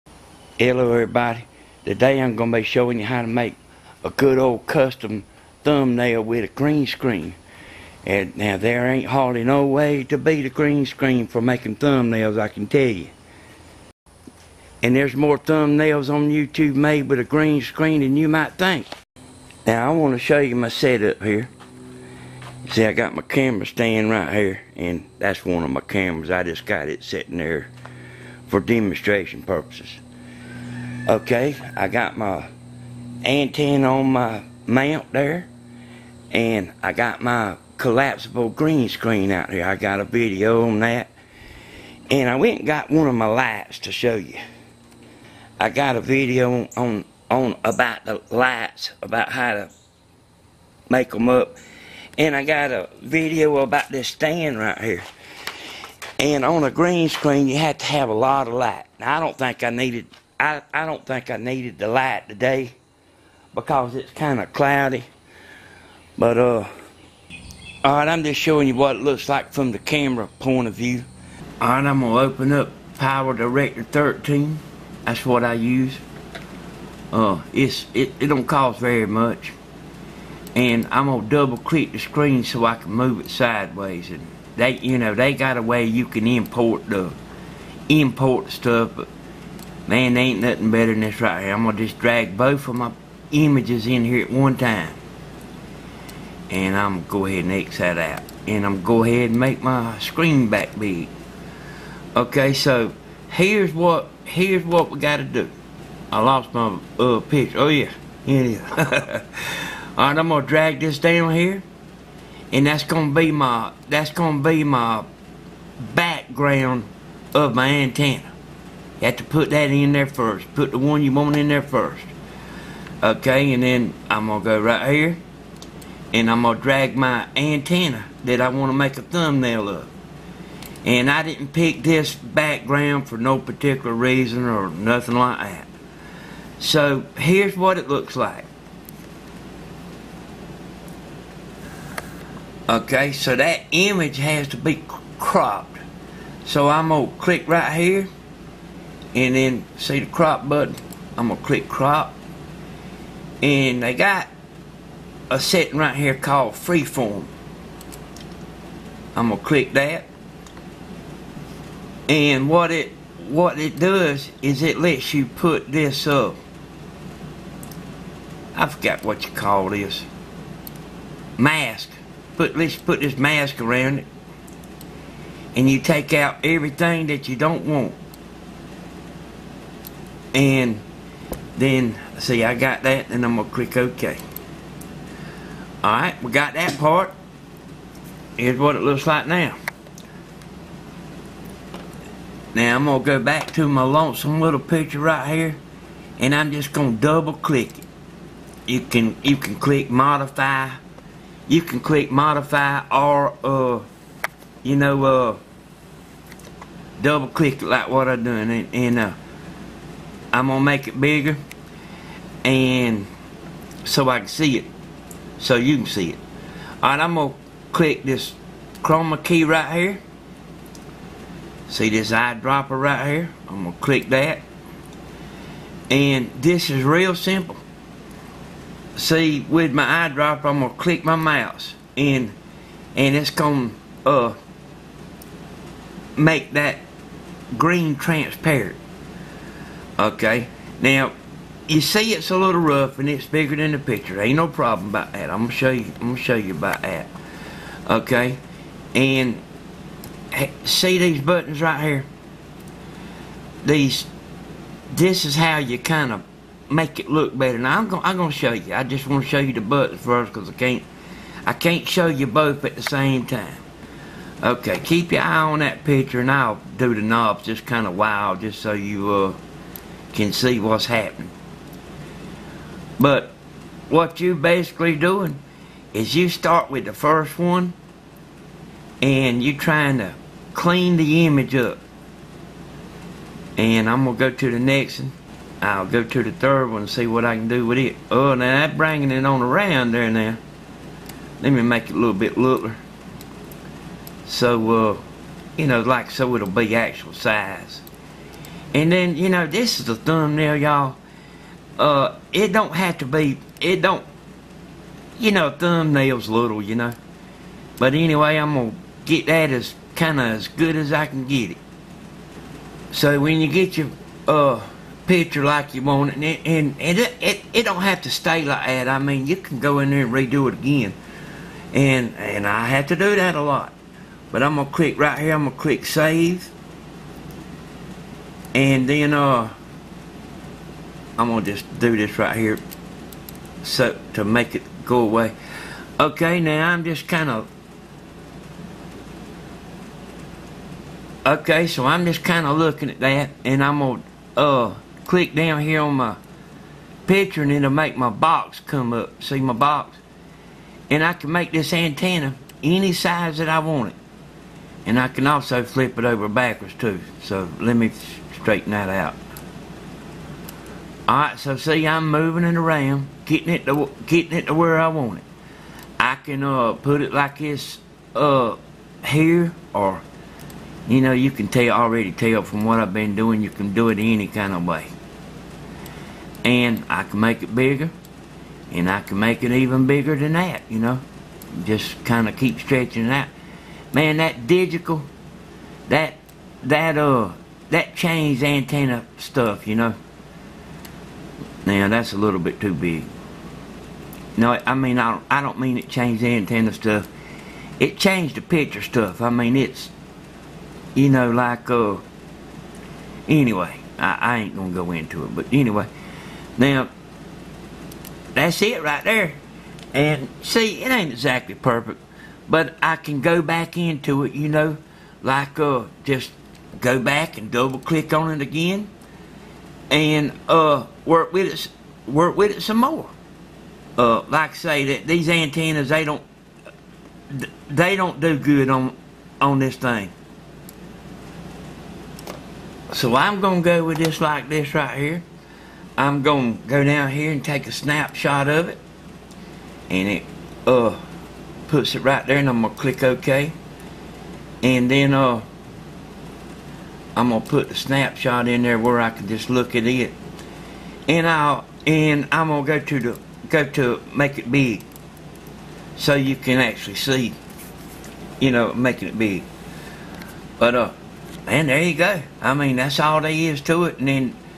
How To Make Custom Thumbnails on Youtube With a Green Screen
In this video I show you How To Make Custom Thumbnails on Youtube With a Green Screen  backdrop. It is very  simple to do. Create custom thumbnails with software. I use PowerDirector 13 software. Get the most out of YouTube with custom thumbnails.